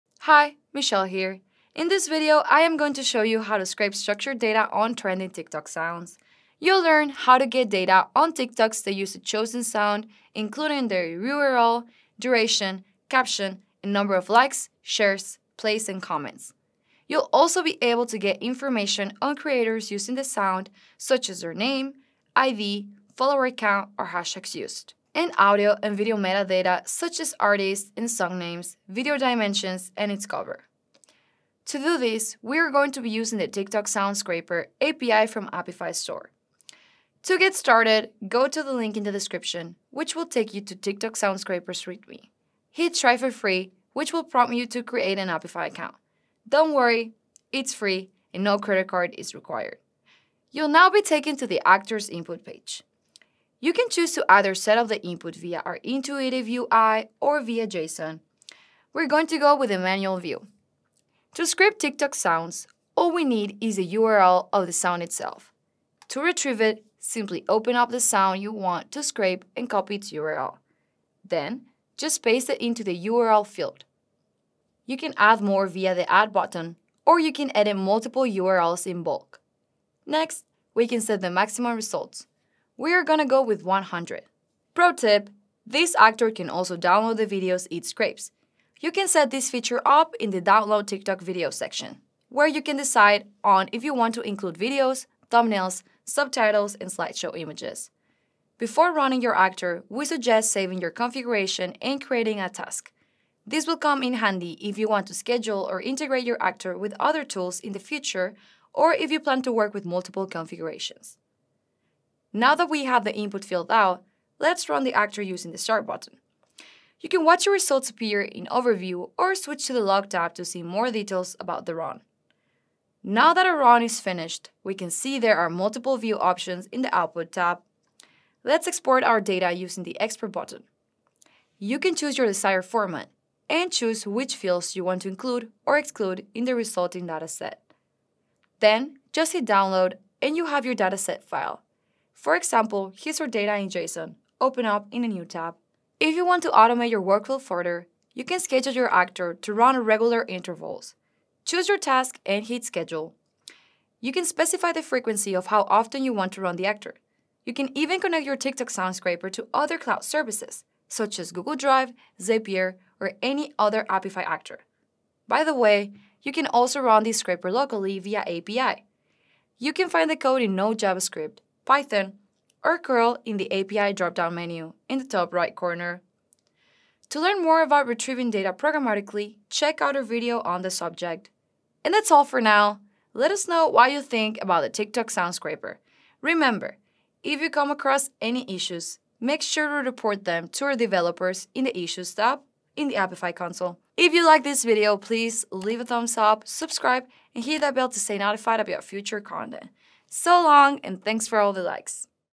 How to Scrape TikTok Sound Data - TikTok Sound Scraper API Tutorial
See how you can scrape data from TikTok sounds with TikTok Sound Scraper from Apify Store 🛍️  After inputting a TikTok sound, this Actor can help you scrape video IDs, text, author, music metadata, plays, comments, URLs, and much more 🚀

*Why scrape TikTok sounds? 🎵*
✨ Conduct sentiment analysis and find authentic product reviews
👩‍🔬 Obtain data for research and social listening experiments
🦠 Analyze trends, viral campaigns and content of specific creators
🥸 Identify unverified content and inaccurate information with high public value
📚 Prepare for or analyze a TikTok marketing campaign

*How to scrape TikTok Sounds 🧑‍🏫*
Step 1. Find the TikTok Sound Scraper on Apify Store
Step 2: Click ‘Try for free’
Step 3: Input a sound link
Step 4: Fine tune the input
Step 5: Start the Actor and get your data!

*Timestamps ⌛️*
00:00 Introduction
00:51 Input
01:52 Run
02:08 Export
02:26 Scheduling
02:37 Integrations
02:45 API
03:02 Like and subscribe!